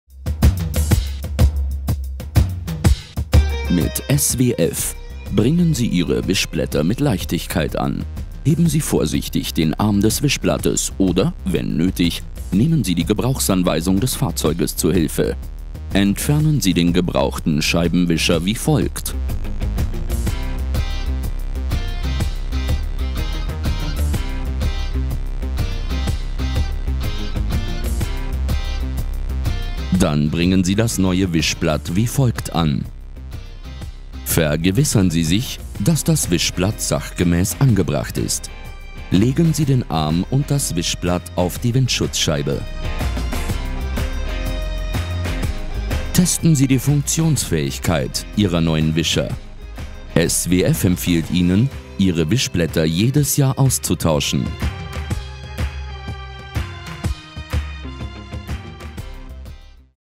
SWF Visioflex - Montage eines Scheibenwischers - Anleitung Typ 10
Mit SWF montieren Sie Ihre Wischblätter mit Leichtigkeit!
1-Heben Sie den Wischerarm an.
2-Entfernen Sie das alte Wischblatt.
3-Befestigen Sie das neue Wischblatt.
4-Versichern Sie sich, dass das Wischblatt richtig angebracht ist.
5-Legen Sie den Arm und das Wischblatt zurück auf die Windschutzscheibe.
6-Testen Sie, ob das neue Wischblatt korrekt wischt.

SWF empfiehlt den jährlichen Austausch der Wischblätter!